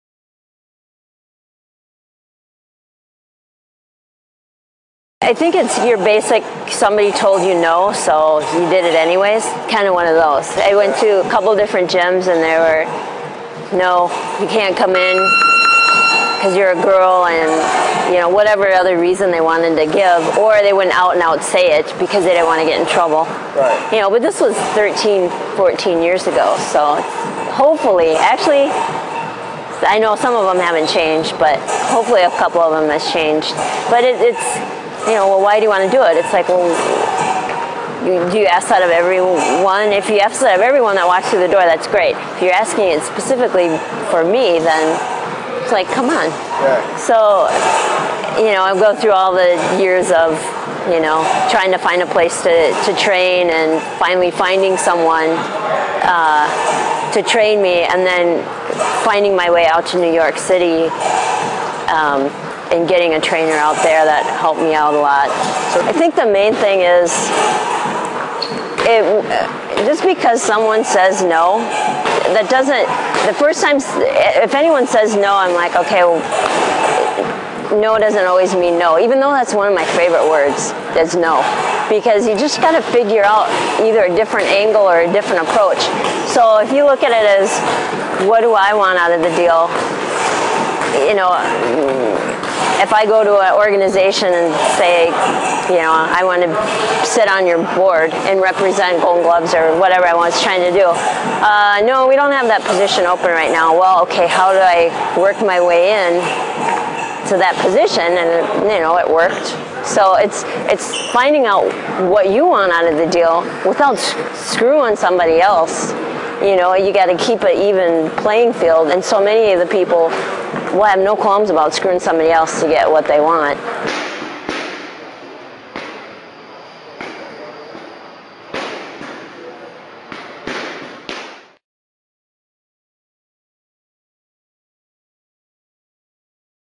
How To Start A Boxing Gym (as a Woman)
Lisa Bauch is the owner of Uppercut Boxing Gym, in Minneapolis - the first woman owned and operated boxing gym in the US. Learn how to start a boxing gym.